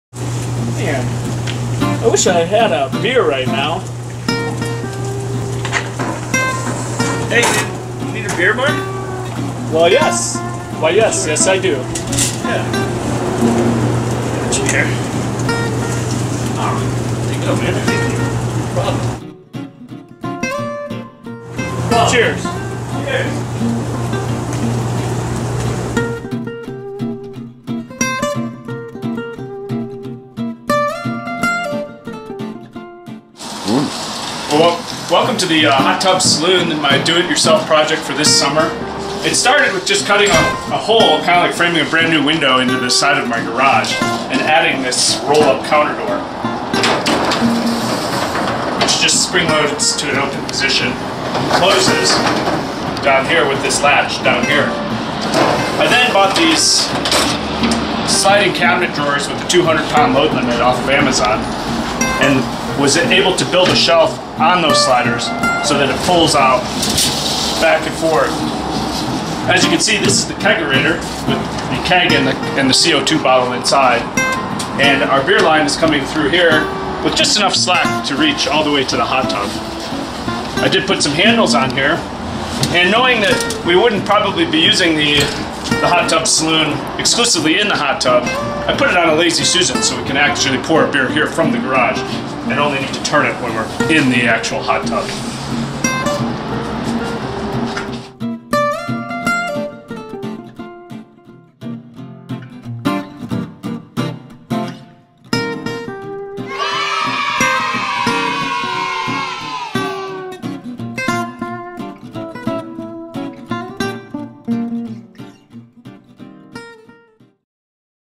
Hot Tub Saloon Final Cut
DIY Project 2016 - Building a hot tub bar!  Next to the hot tub, I installed a roll-up counter door, and a sliding bar with a lazy susan on it.   Now I can pour myself a fresh draft beer right from the hot tub!